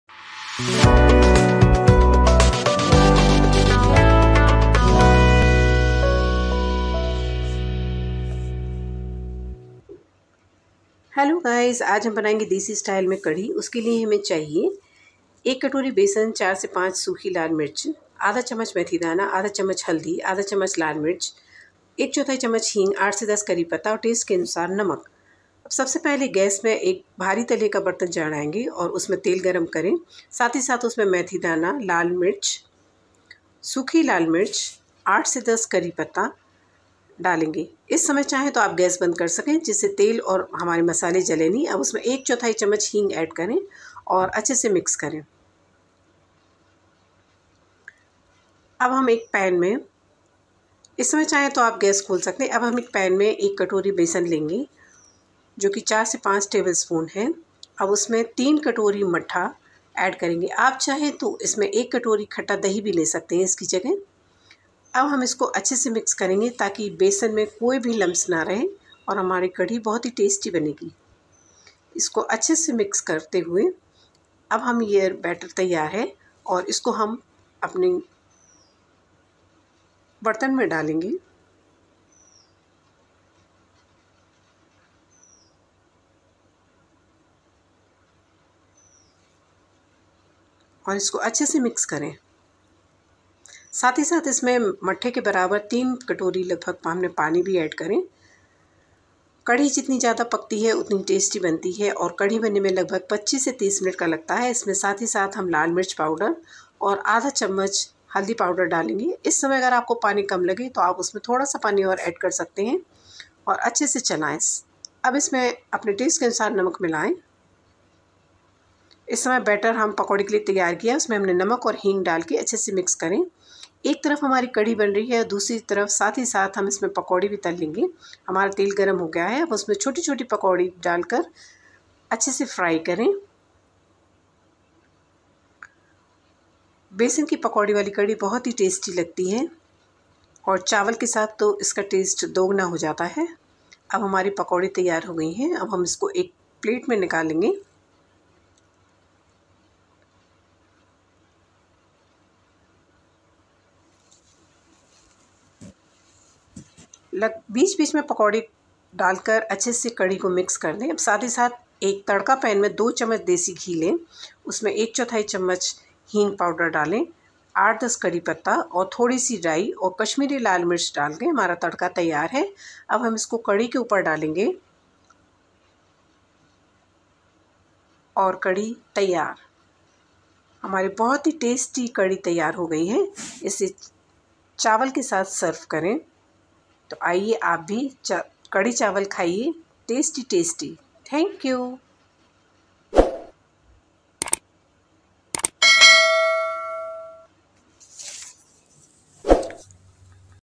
देसी कढ़ी | pakodhe kadhi | kadhi pakoda recipe | kadhi chawal | Indian curry recipe in hindi
Watch how to make kadhi in unique and easy way...